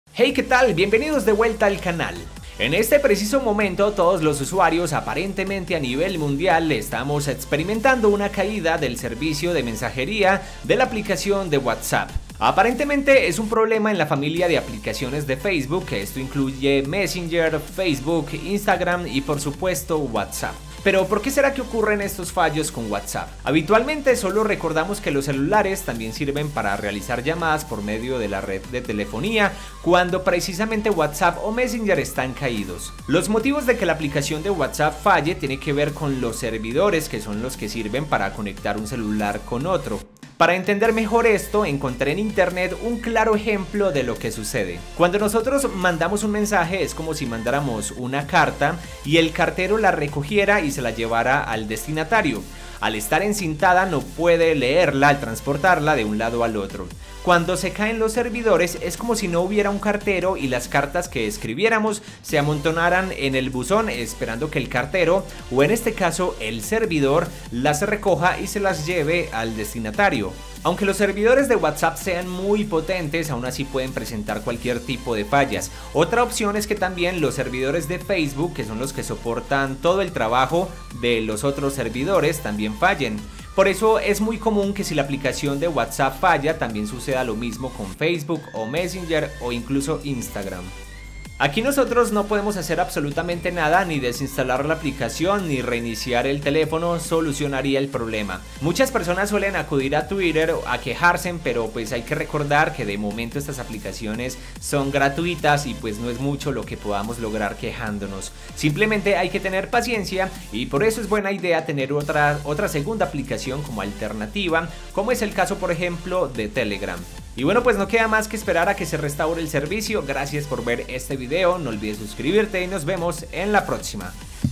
¡WhatsApp CAIDO! ¿POR QUE SE CAE Y COMO SOLUCIONARLO?
whatsapp no sirve o esta caído en estos momentos aunque intermitentemente ya esa funcionando en varios paises, por que se cae whatsapp? por que los servidoresd e whatsapp fallan tanto y como solucionar problemas de whatsapp, aunque les adelanto que cuando se cae whatsapp es poco lo que podemos hacer.